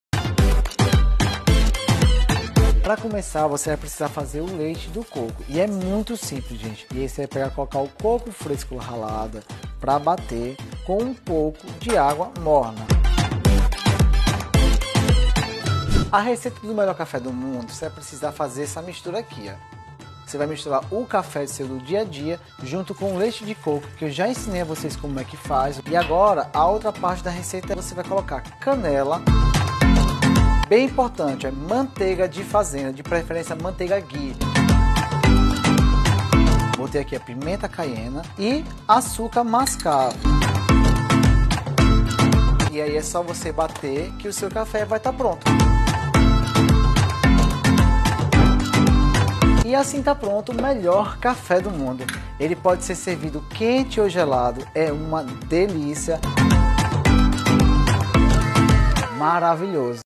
O MELHOR CAFÉ DO MUNDO
Melhor Café do Mundo 😊

Já ouviu falar do café a prova de balas?

Ele é o queridinho da galera fitness por ser delicioso, super funcional e faz bem à saúde! ☕

Conheça e faça a receita desse café e se apaixone junto com a gente! ❣